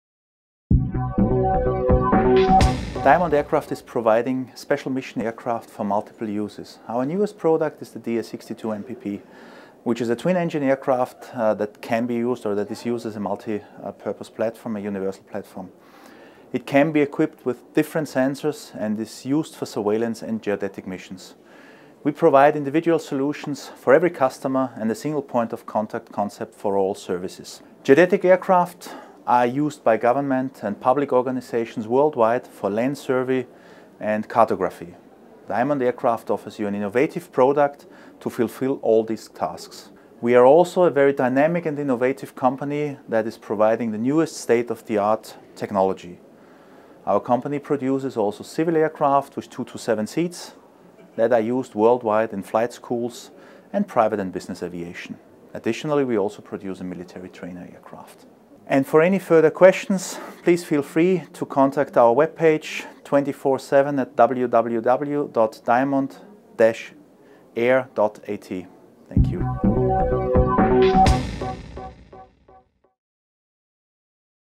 INTERGEO 2017 Shortfact | Diamond Aircraft Industries GmbH
Alexander Maierhofer, Diamond Aircraft Industries GmbH at INTERGEO 2017 in Berlin. Save the date: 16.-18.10. Frankfurt INTERGEO 2018 .